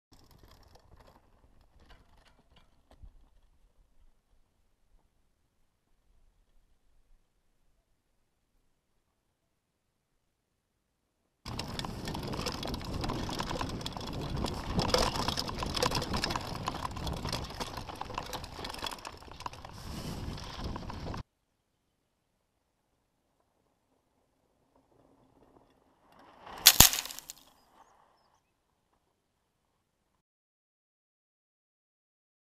Szybka Akcja. Altitude Movies
szybki zlepek kilku fajnych ujęć z dzisiejszego wypadu w okolicy skrzycznego :)